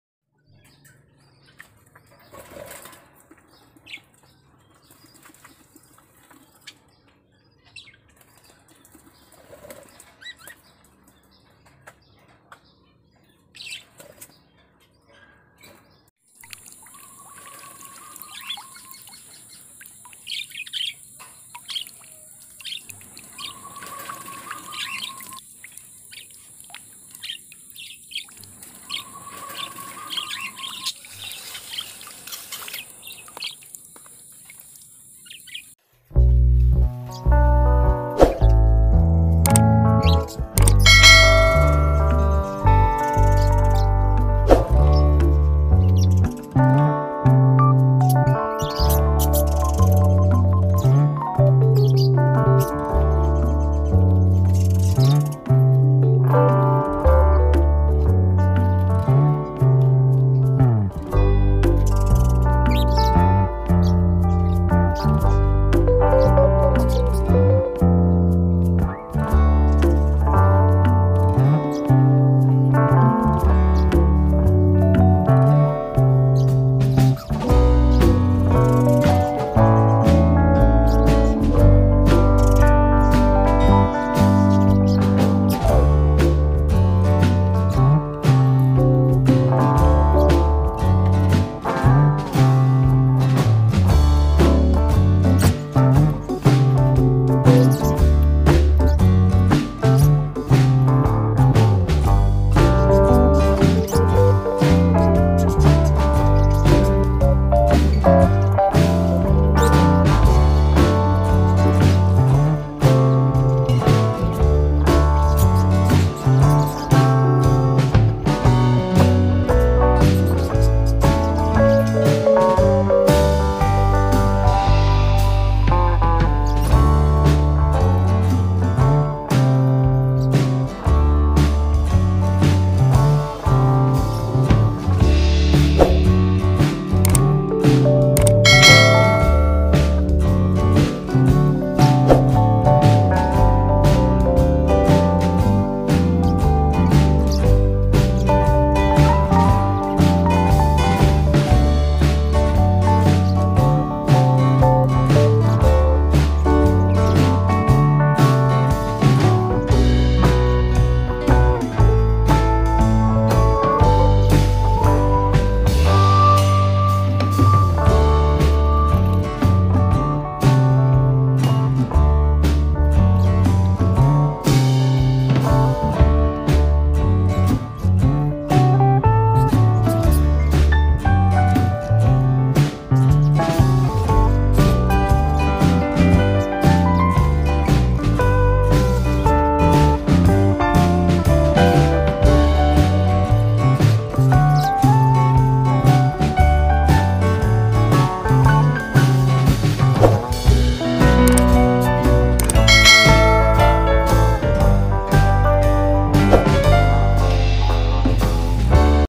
Supaya tidak stres di cuaca panas siang hari || FALK mandi
Supaya burung falk kita tidak stres di cuaca siang yang panas.
maka sediakan cepuk untuk burung falk mandi atau bisa di semprot.
kalau cuaca panas, burung bisa stres, dehidrasi yang bisa mengakibatkan gagal produksi